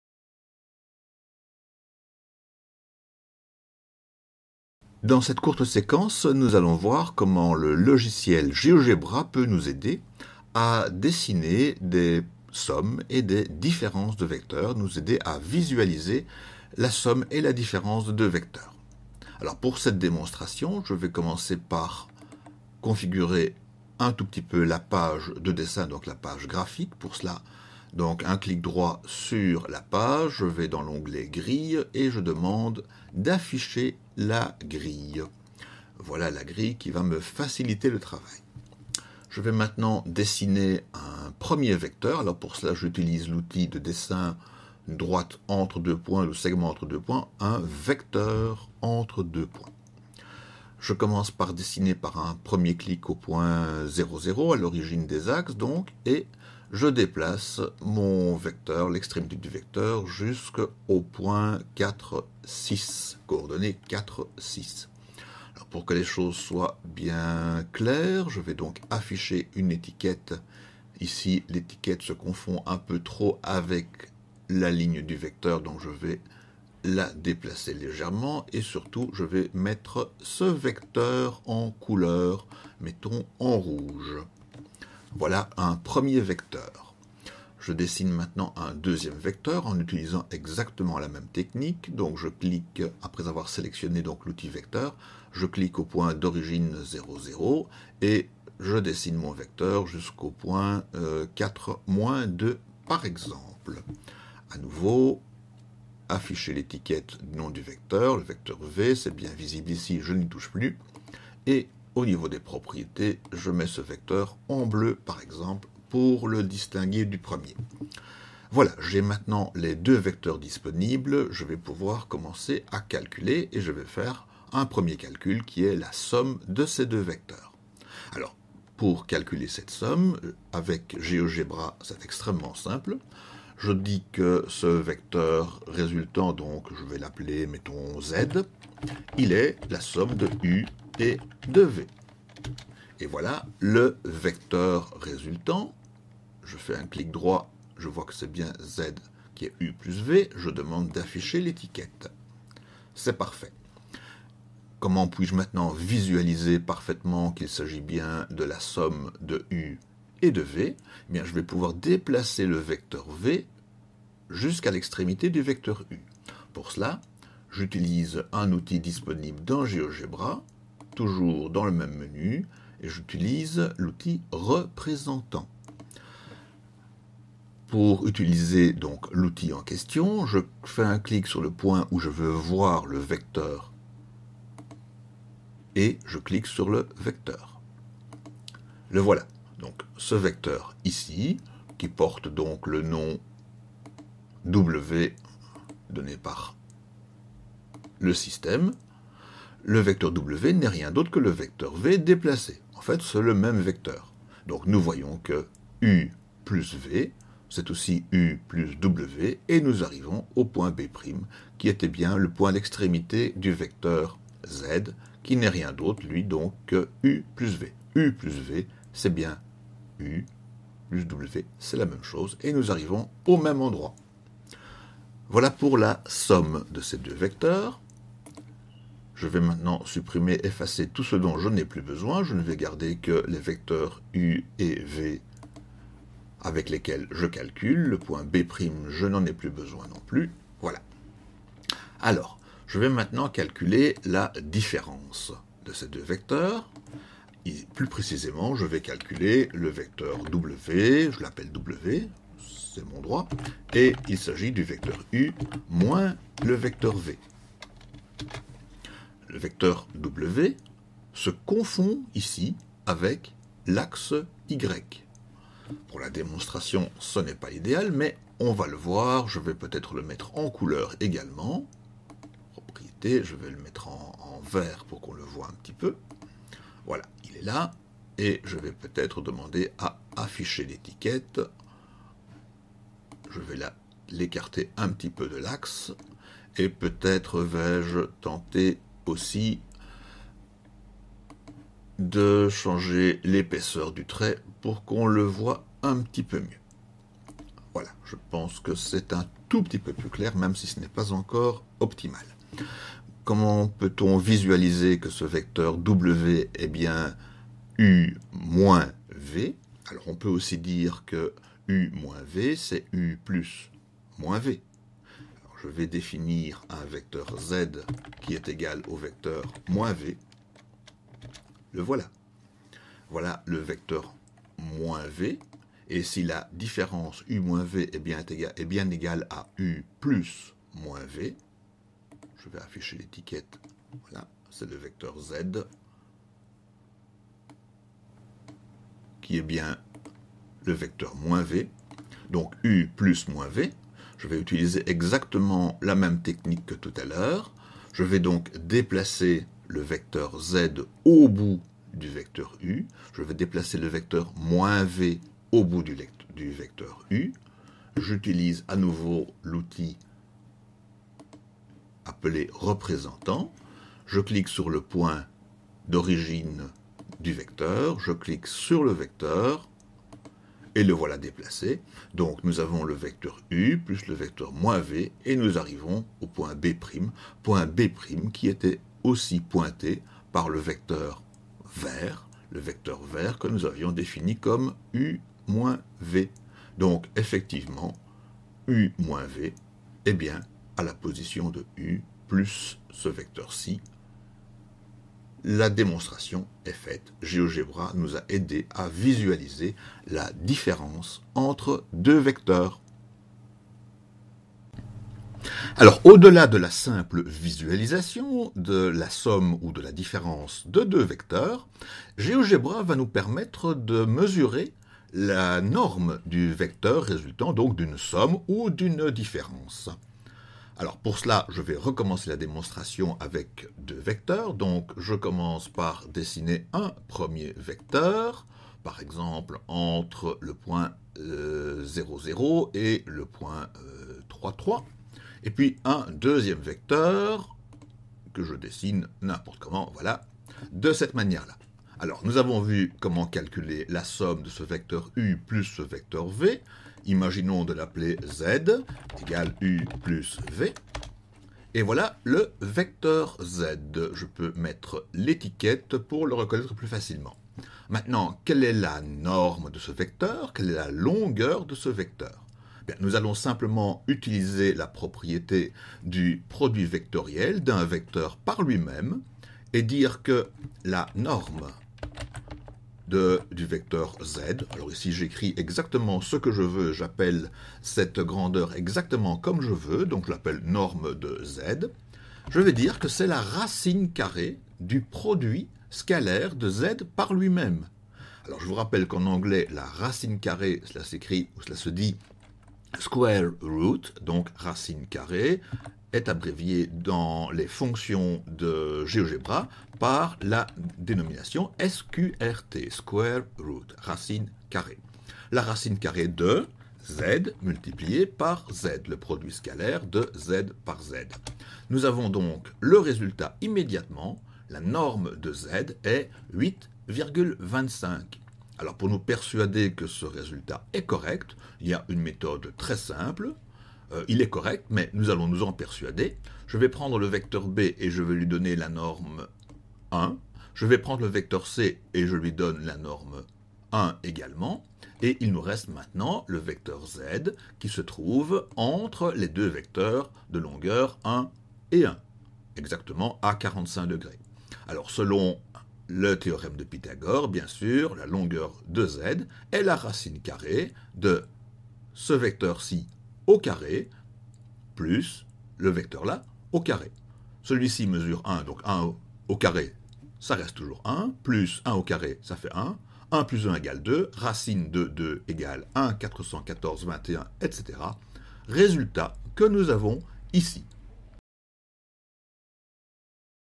Somme et différence de vecteurs avec Geogebra
Somme et différence de vecteurs avec Geogebra; calcul de la norme du vecteur résultant.